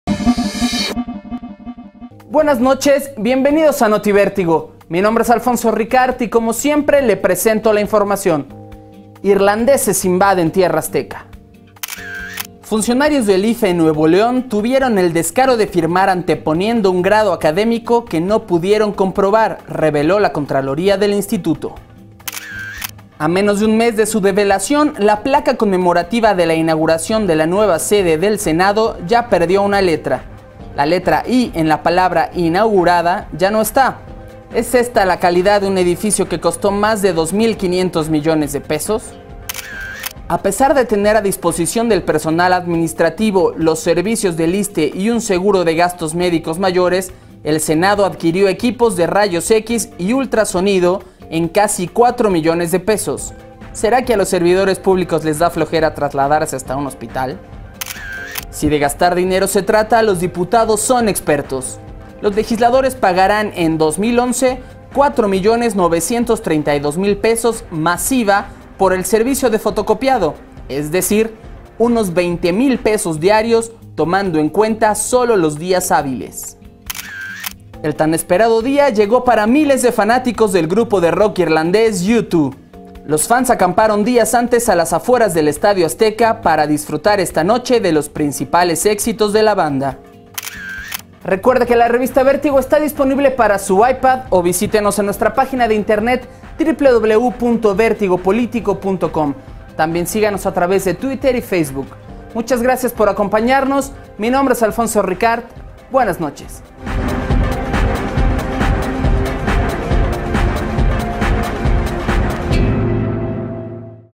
Noti Vértigo 11 de mayo 2011
Con la información destacada del día de hoy 11 de mayo 2011: funcionarios del IFE en Nuevo León firman con otro grado académico, la placa conmemorativa de la nueva sede del Senado ya perdió una letra, el Senado adquirió equipo médico a pesar de contar con servicio del ISSSTE, diputados gastarán 4 millones 932mil pesos por servicio de fotocopiado en 2011, U2 se presenta en México